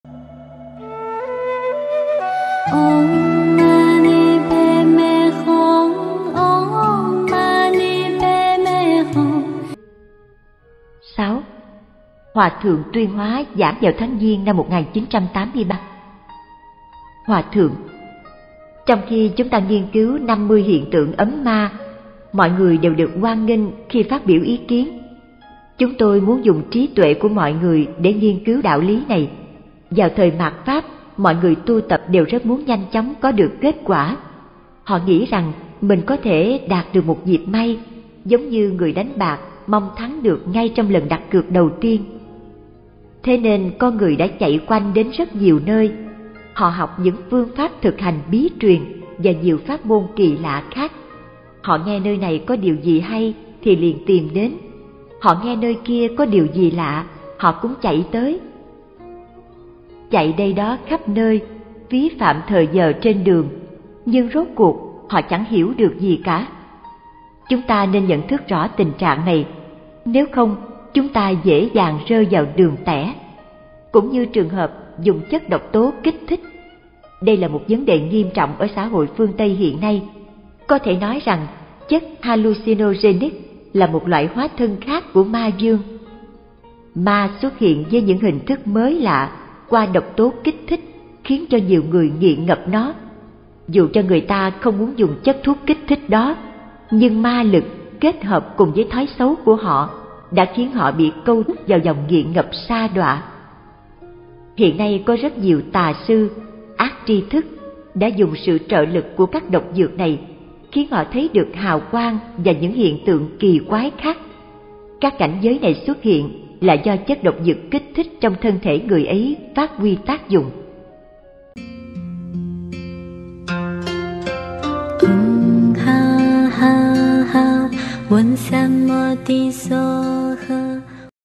Thuốc gây ảo giác Hallucinogenic là do Ma Vương tạo ra | HT Tuyên Hóa giảng
Thuốc gây ảo giác Hallucinogenic là do Ma Vương tạo ra
50 Hiện Tượng Ngũ Ấm Ma - HT Truyên Hóa giảng
Ma xuất hiện với những hình thức mới lạ, qua độc tố kích thích khiến cho nhiều người nghiện ngập nó.
Hiện nay có rất nhiều tà sư (ác tri thức) đã dùng sự trợ lực của các độc dược này khiến họ thấy được hào quang và những hiện tượng kỳ quái khác.